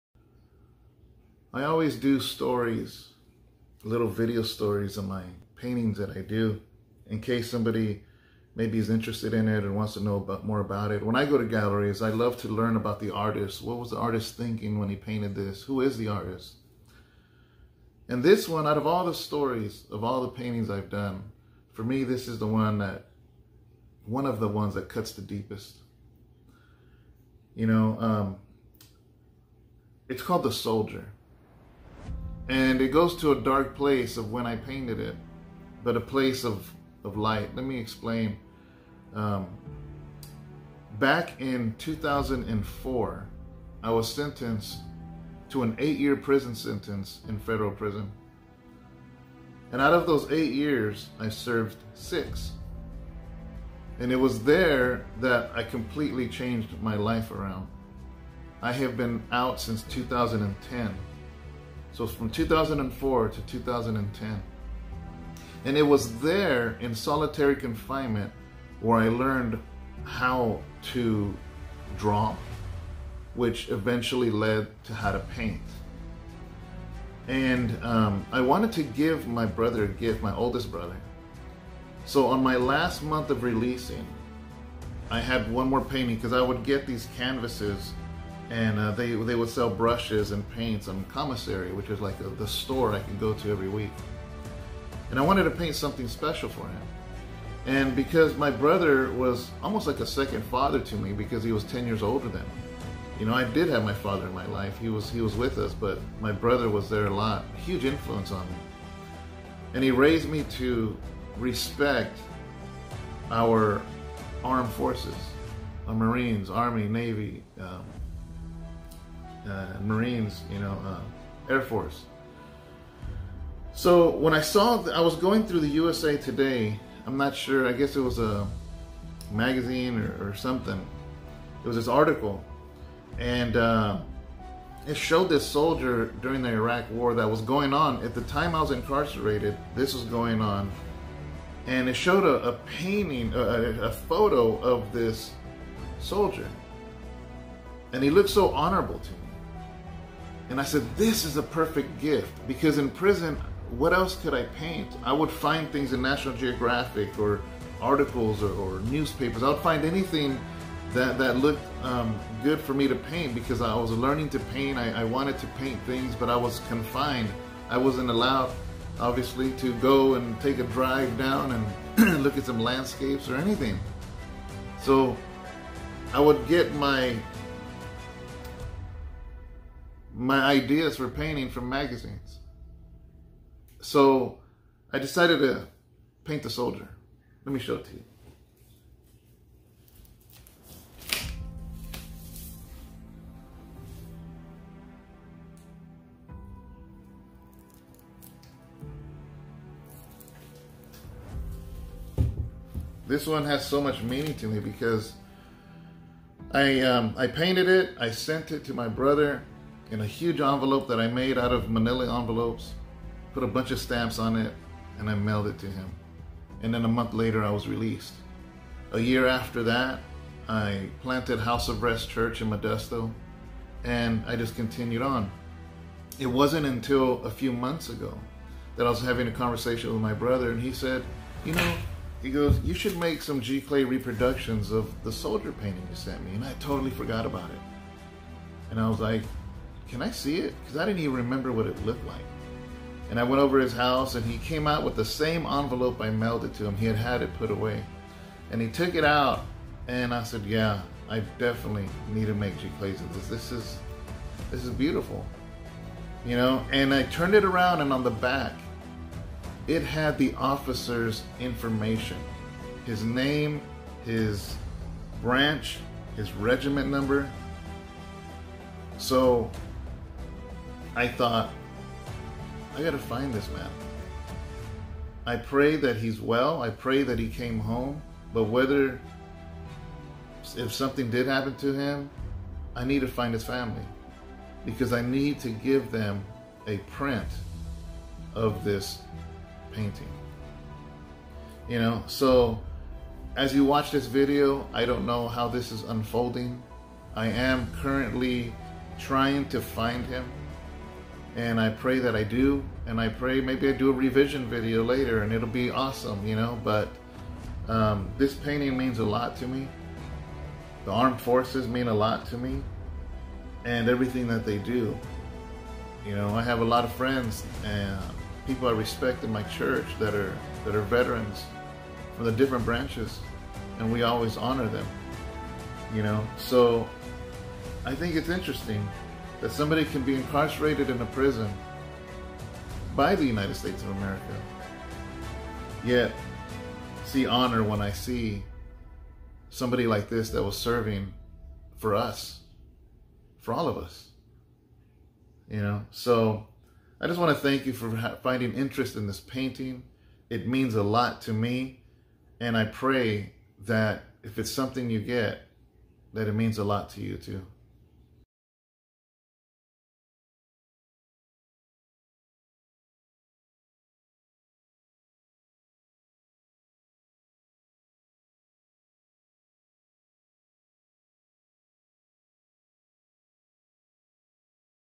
My last painting before releasing prison - the Soldier
I learned to paint in federal prison. I began with pencil drawing while in Solitary Confinement in Santa Rita 2005. During my last 2 years of incarceration I was sent to Atwater federal prison camp and had access to brushes, paint and canvas. I released in 2010. Everything I learned was with limited resources. I hope this can inspire you to pick up a brush and paint.